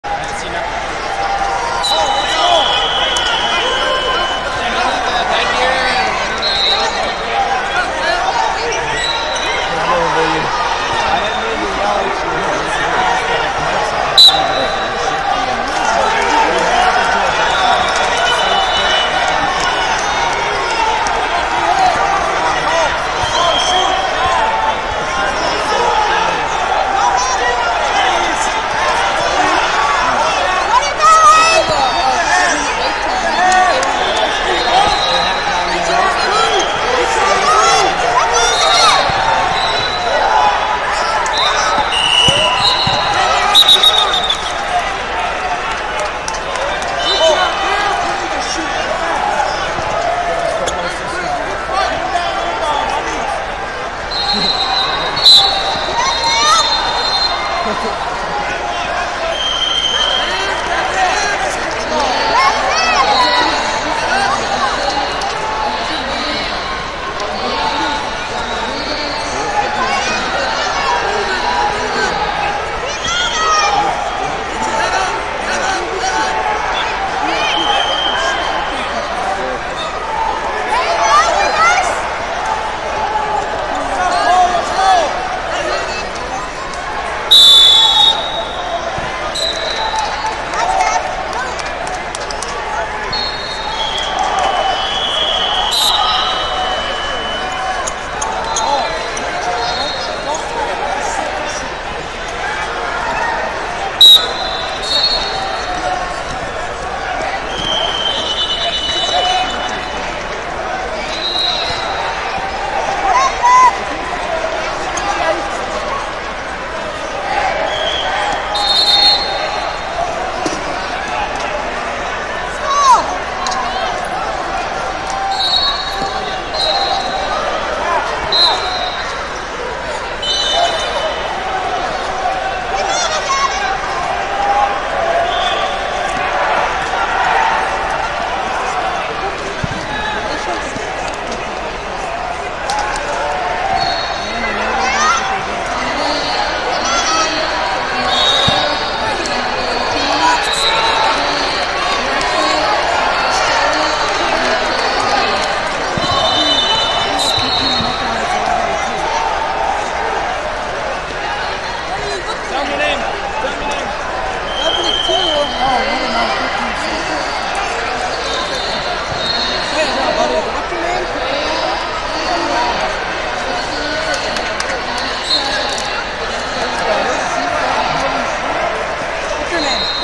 67 Lbs Cons. Round 2 - Cole Cuyler, Milton Monsters Wrestling Club Vs Gavin Decker, Team Nazar Tra
Flosports-partner-access/2023 Folkstyle Tour Of America Dominate In The Dells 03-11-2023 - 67 Lbs Cons. Round 2 - Cole Cuyler, Milton Monsters Wrestling Club Vs Gavin Decker, Team Nazar Training Center Ad94